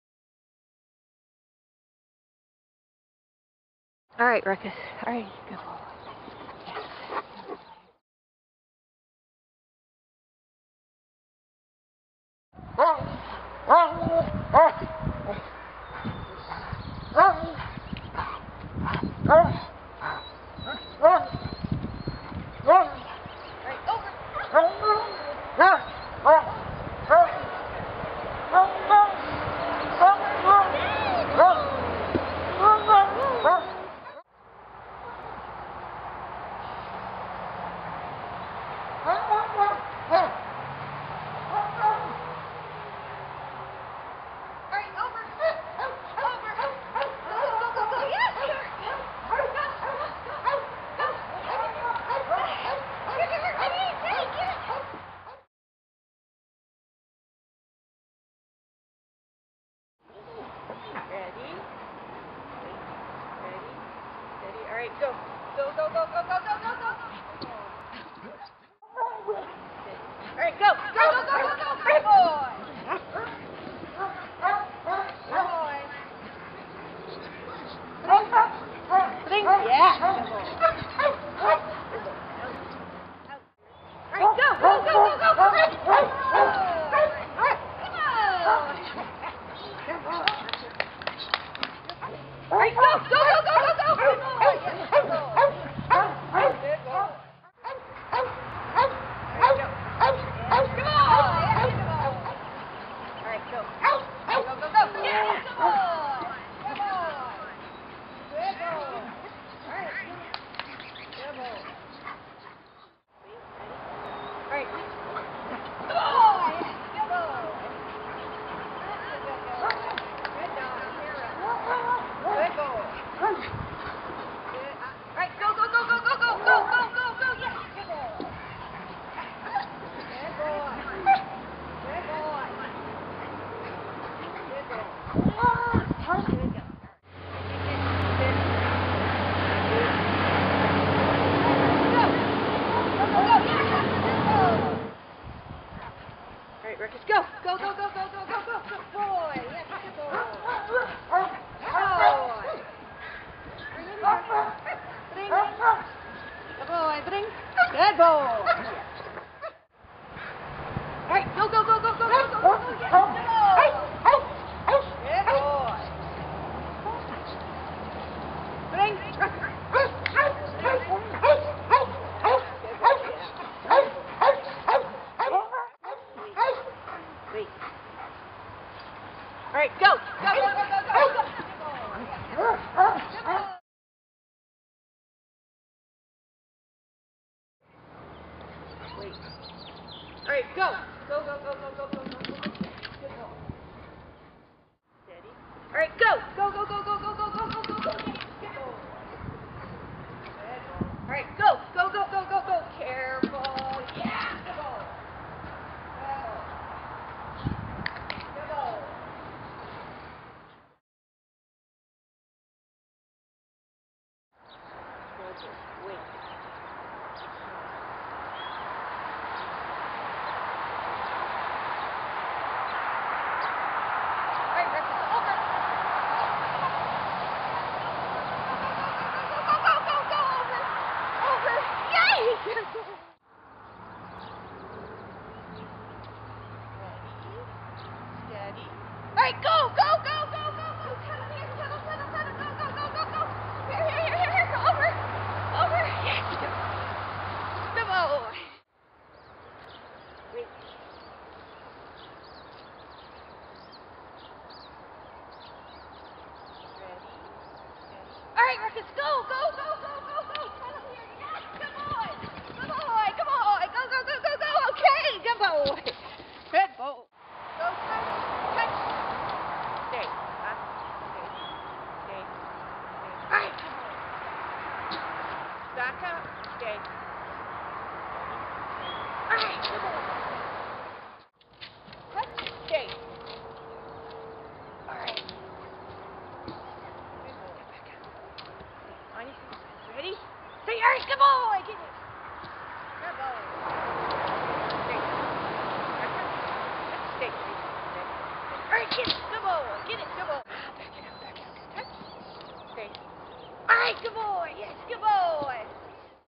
Foundation agility Exercies with Ruckus
3 year old male Briard Ruckus works on some foundation exercise for agility.